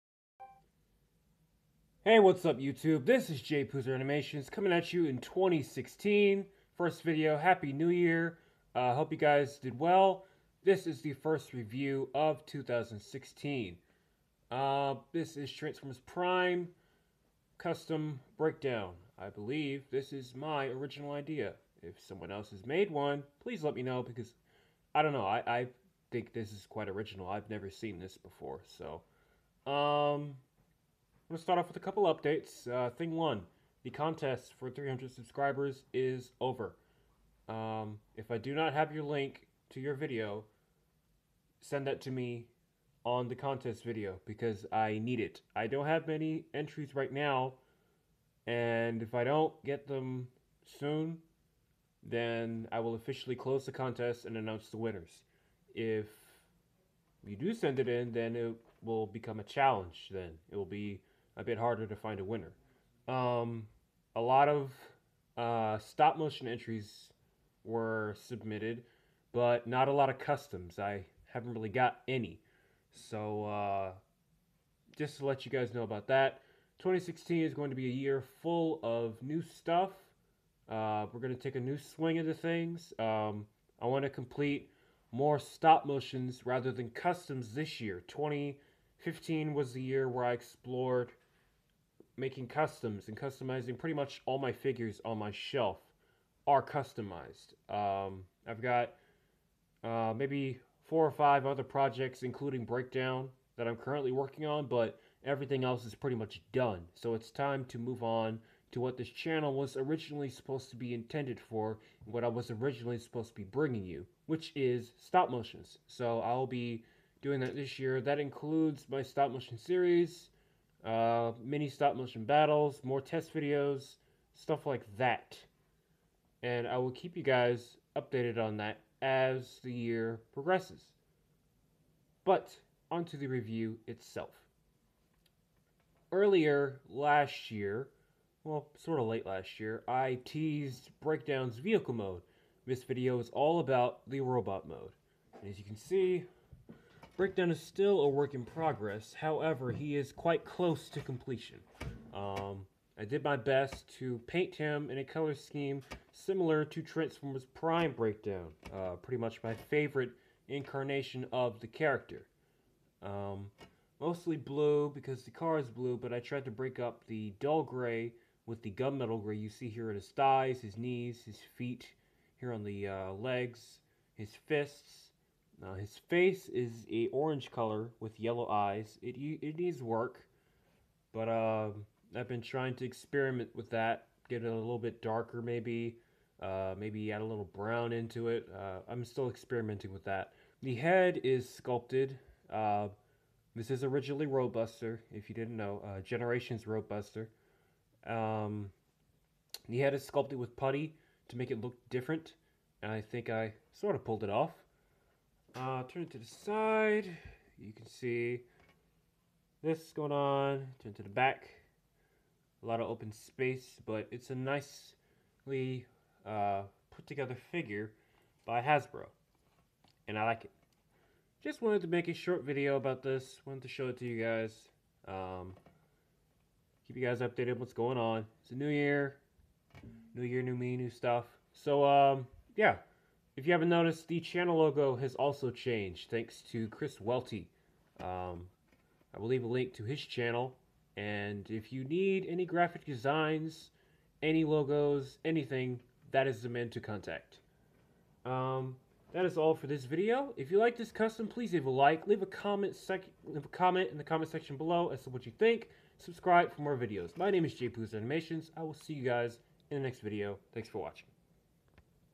Transformers Prime BREAKDOWN Custom Part 2/Updates (GENERATIONS ROADBUSTER)
This video is all about Breakdown's robot mode! Enjoy!